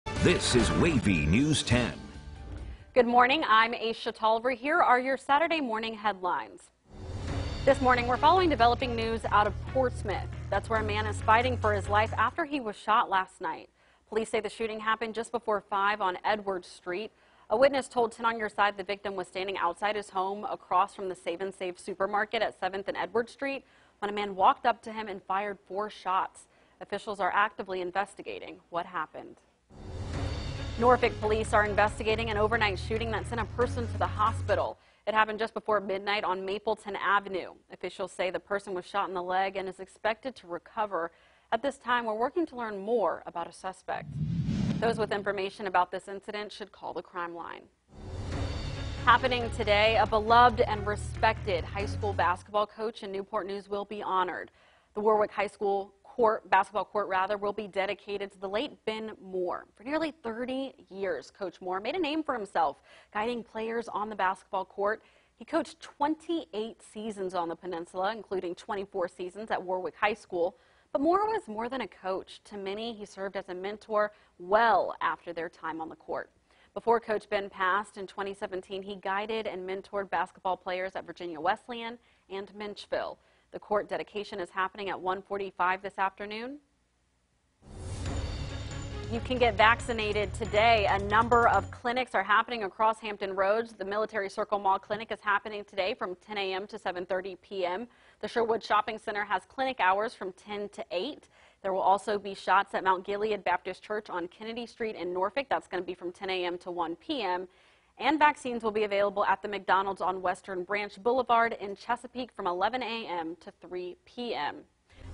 Top Local Headlines | Nov. 13, 2021
WAVY News 10's Aesia Toliver has the latest headlines for Nov. 13, 2021.